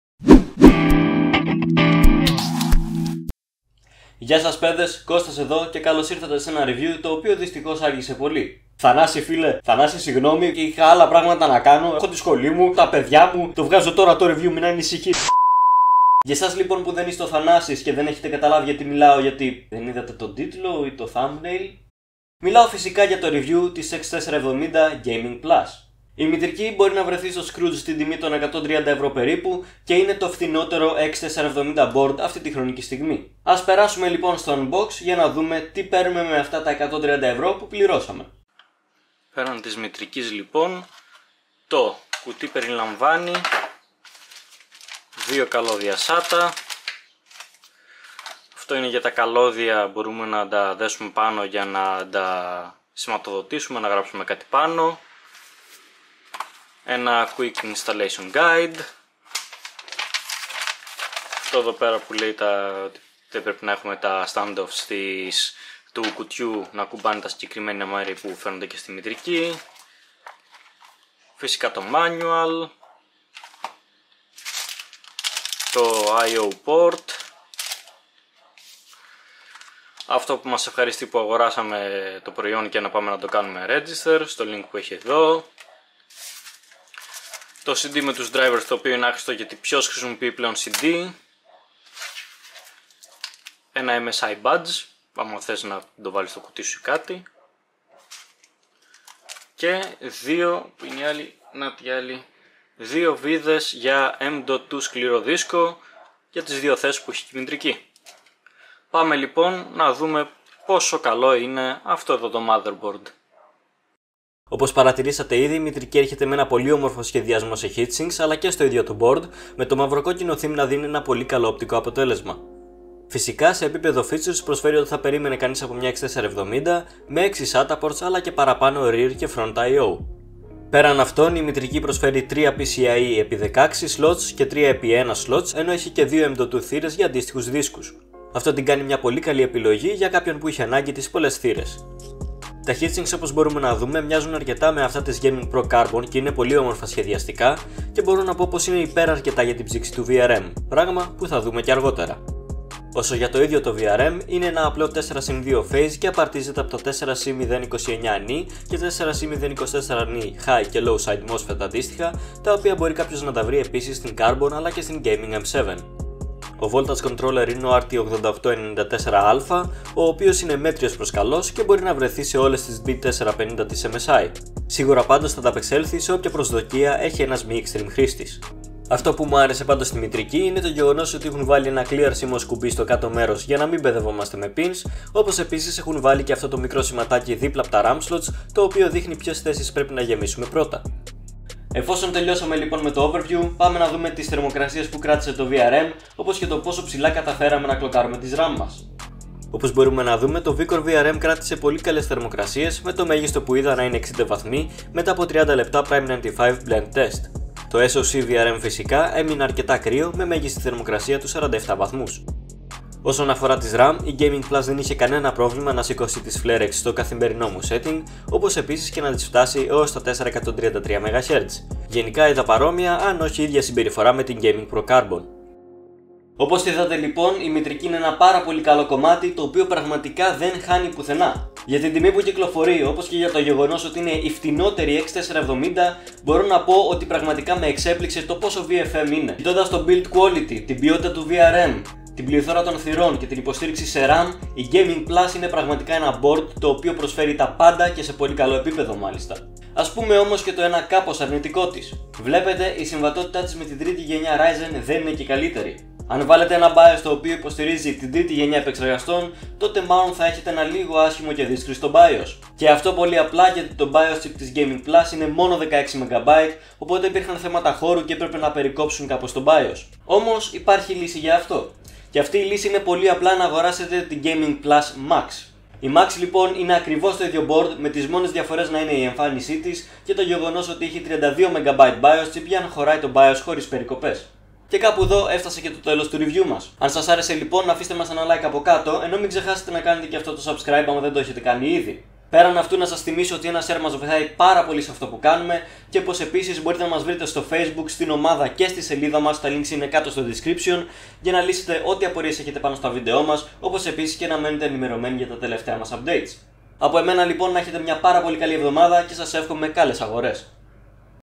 MSI X470 Gaming Plus Review - H φτηνότερη X470 της αγοράς!
Η φτηνότερη X470 της αγοράς είναι αρκετά καλή για να την αγοράσει κάποιος, ή υπάρχει μόνο για να πάρει τον τίτλο του "φτηνότερου";
Αυτό ερευνούμε στο σημερινό review!

Bρείτε μας στα socials @The TechLads αλλά και στο website μας για news, γραπτά reviews και πολλά άλλα!

Το site του συνεργάτη μας dwrean.net!